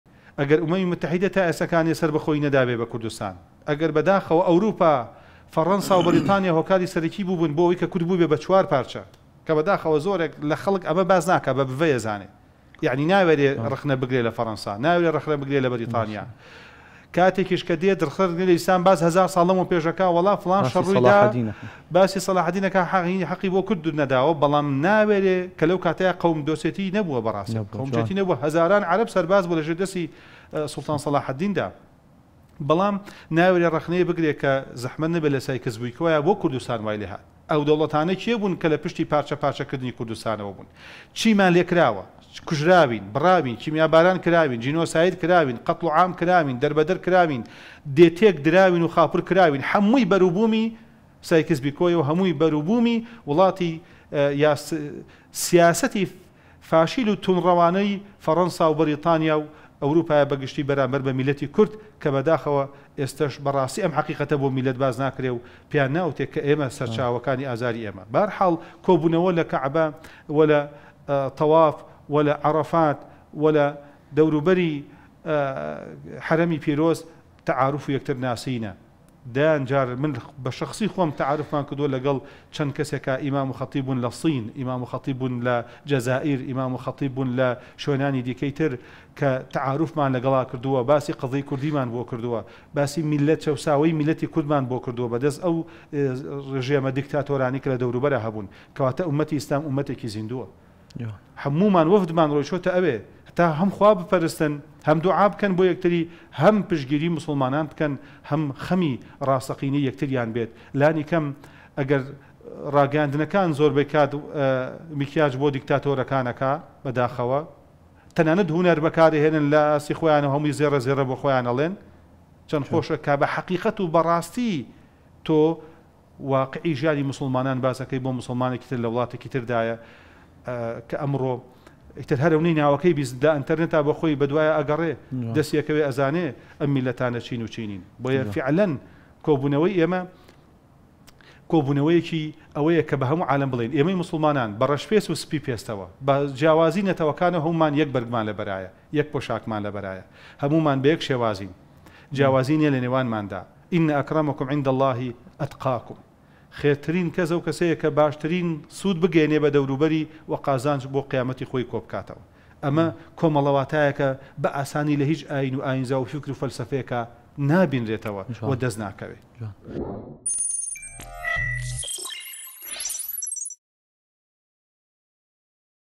کێش وەک کوردستان لەناو ئازاردایە؟
کێ هوکاربوو بۆ دابەشبوونی کوردستان؟
کێش لەناو ئازارەکانی خۆیدا تۆمەتباردەکرێت؟